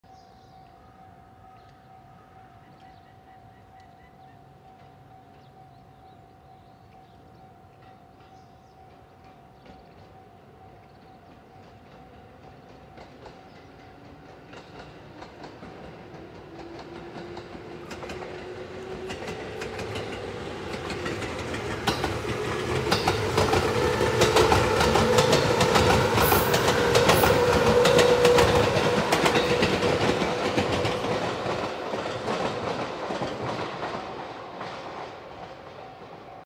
【定刻運行】列車番号523M 普通 213系4両編成 児島行き 妹尾駅を発車！
妹尾駅# 213系
列車番号523M 普通 213系4両編成 児島行きは、定刻運行で、妹尾駅を発車、児島へ向かいました。